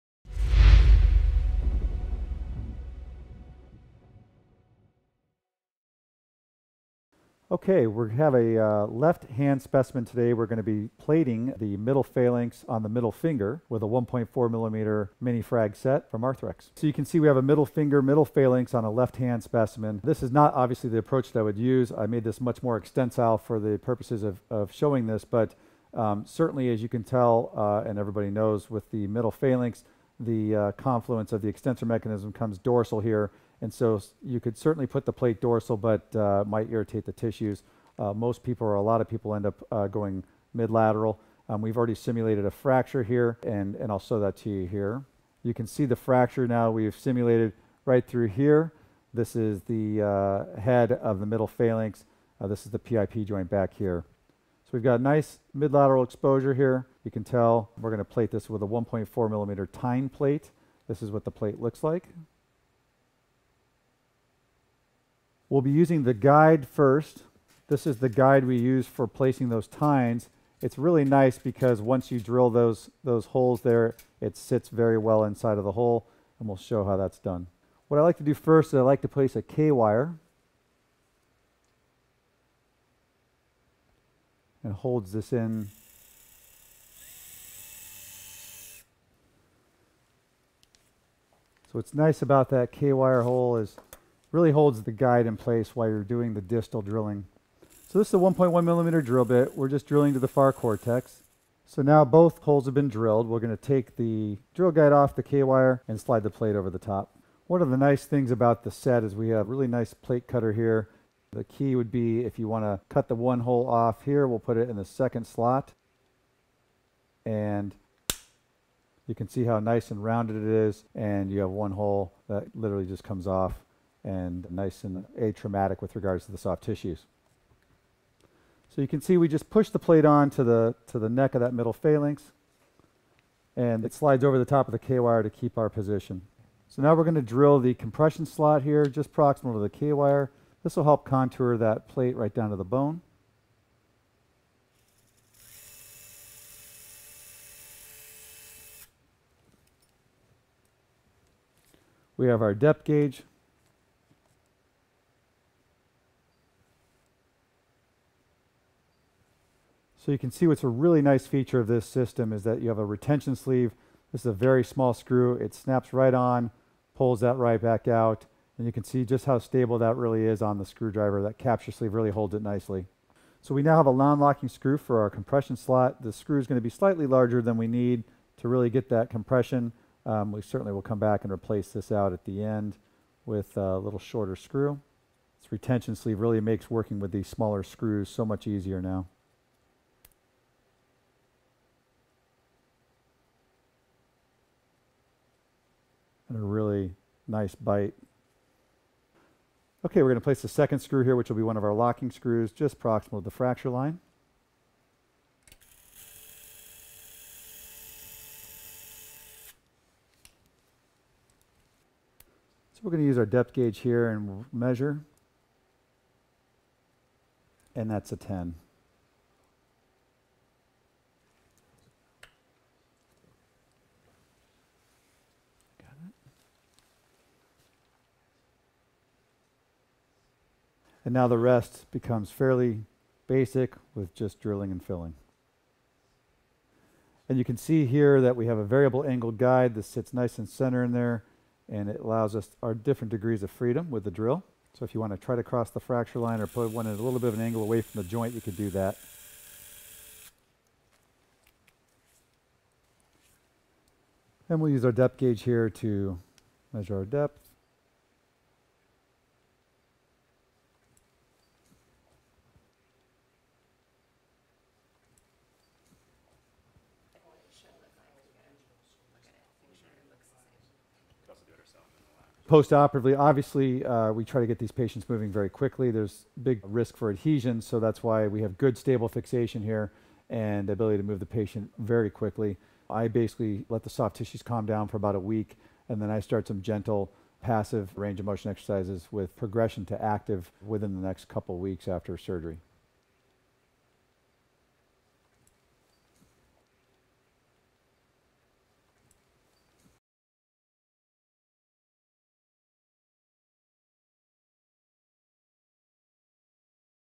Phalangeal Fracture Repair Using the 1.4mm Mini Frag Set for Tine Plate Fixation
Damon C. Adamany, MD (Phoenix, AZ), fixes a phalangeal fracture with the tine plate from the Mini Comprehensive Fixation System (CFS).